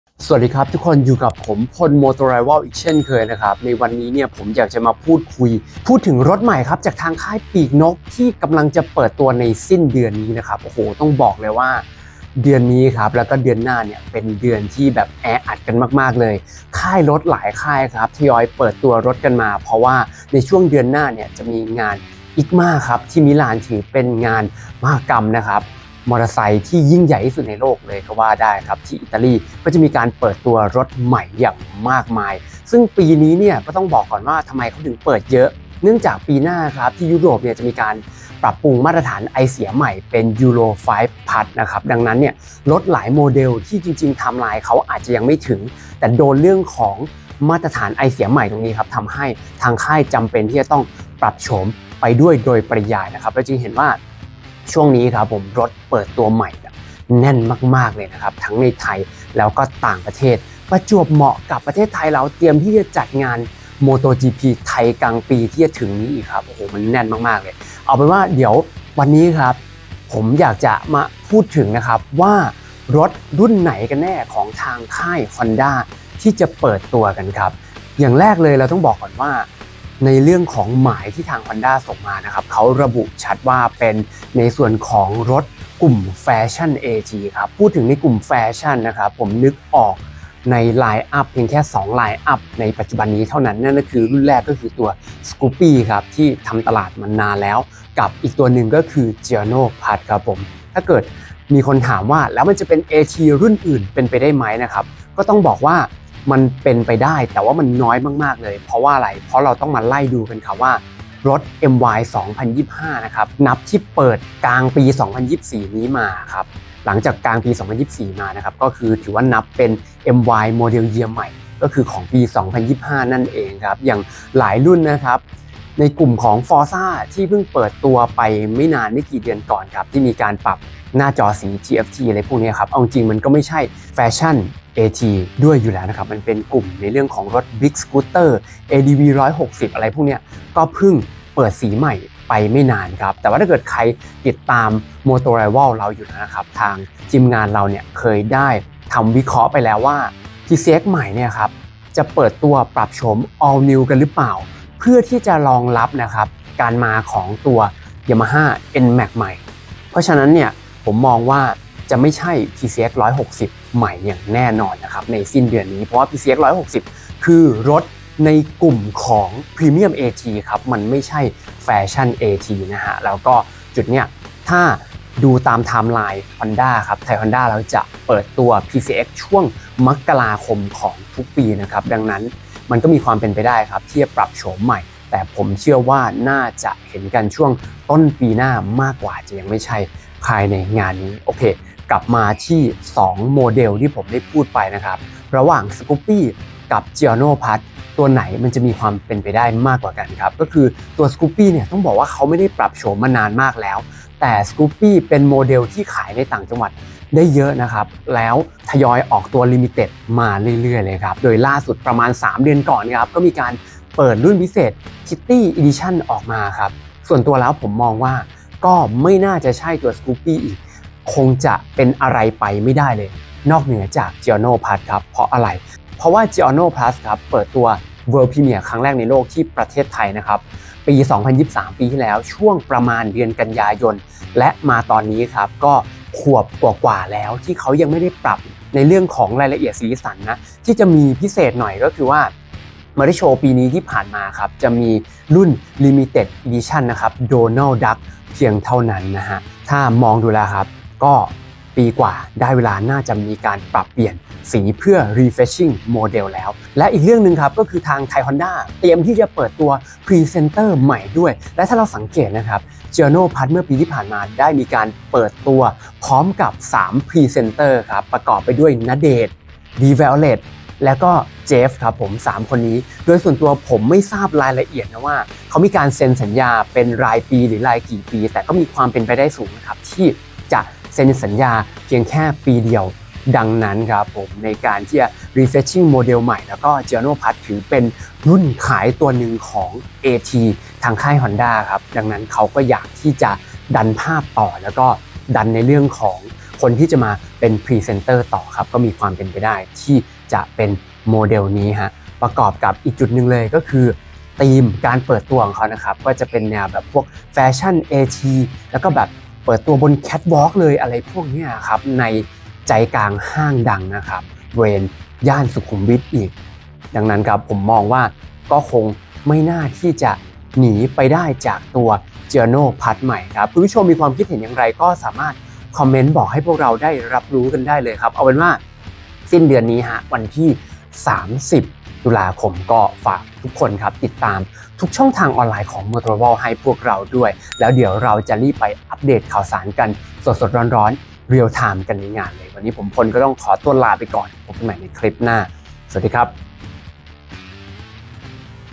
Honda เปิดมอไซค์ อะไรสิ้นเดือนนี้?
0:00 intro
0:11 ทำไมค่ายรถ หลายค่ายเปิดอะไรกันช่วงนี้เยอะ
1:09 Honda จะเปิดรถรุ่นใหม่ อะไร?